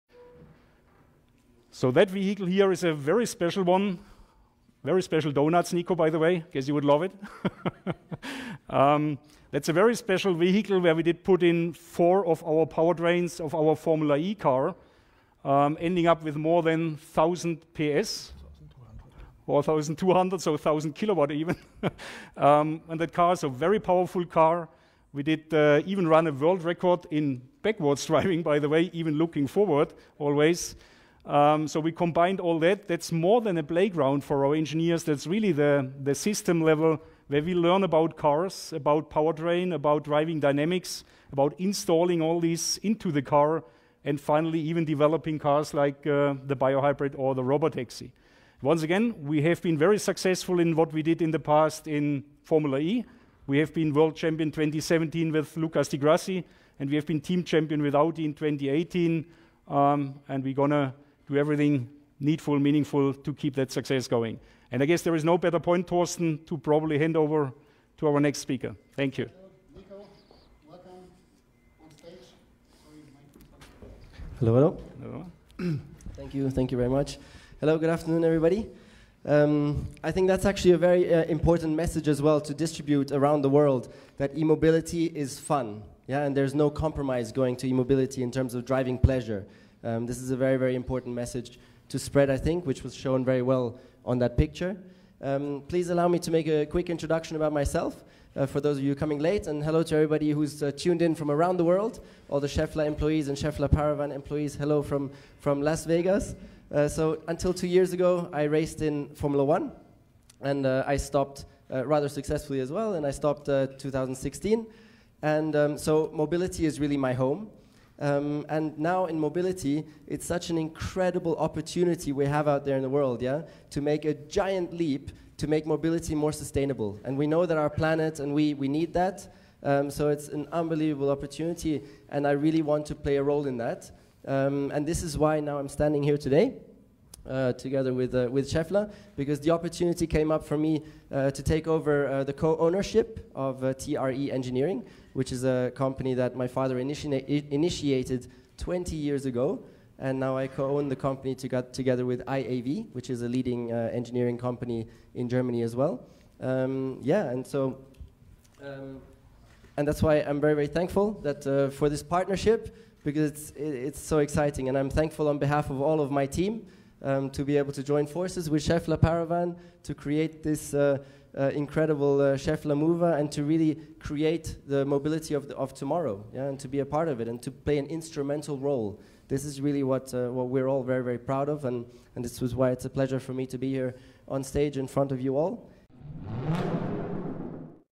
1200HP Audi RS3 Based Schaeffler 4ePerformance CES 2019
Described by Schaeffler as a showcase for how quickly high-performance electric drive technology can be integrated into a series production model, the 4ePerformance can accelerate to 124mph in less than seven seconds — that will make it at least six-tenths quicker than the upcoming Ferrari 488 Pista's time of 7.6sec.

The car’s four electric motors are identical in specification to the one used in the 2016/17 ABT Schaeffler FE01 Formula E car, which was driven by Lucas di Grassi and won the series title before the racing outfit was taken over by Audi (hence the use of an RS3 for the concept). Each motor produces up to 295bhp and 236lb ft of torque.

The two motors of each axle are packaged tightly within a single gearbox housing to create a pair of electric twin axles, but with each motor connected to its own wheel via a straight-cut gear, it enables four-wheel torque vectoring. Power for the system is provided by 64kWh batteries.